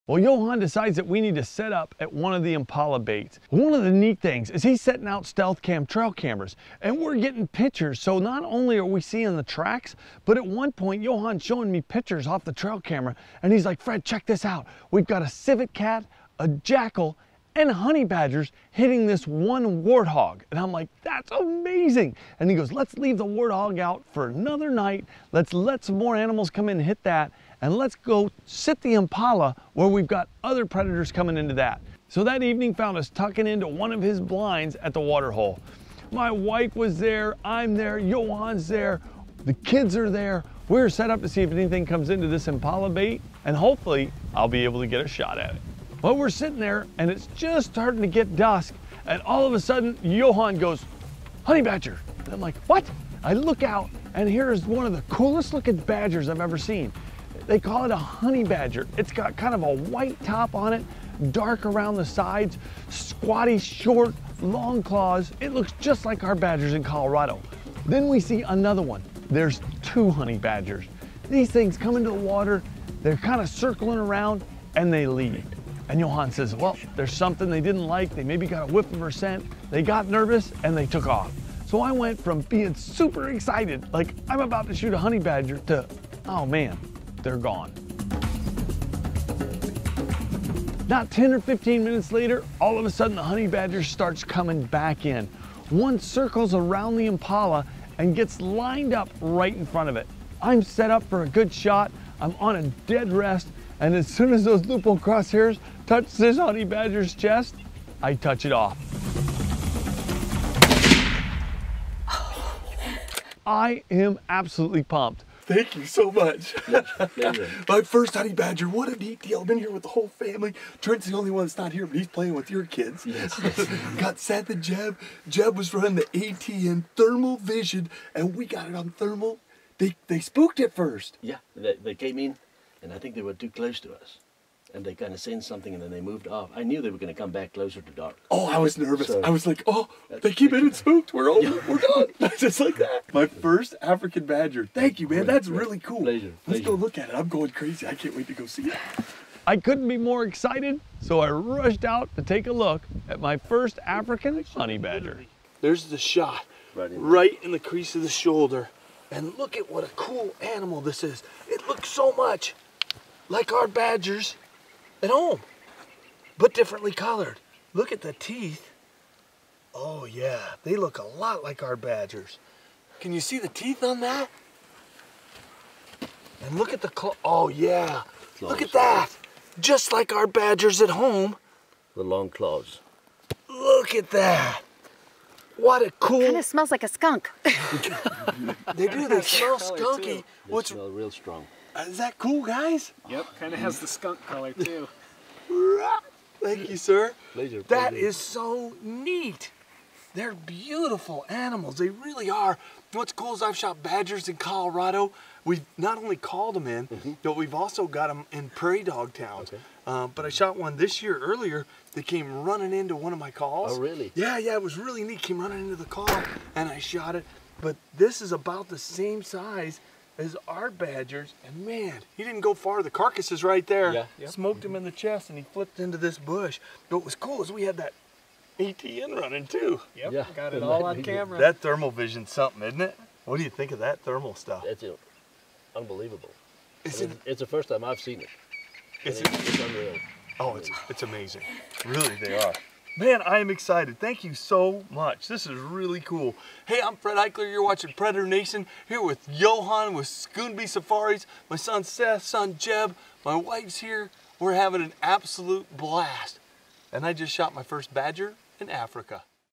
Fred Eichler takes a honey badger in Africa
Fred takes an African honey badger while in Africa with the family.  The shot was also captured on the ATN thermal vision.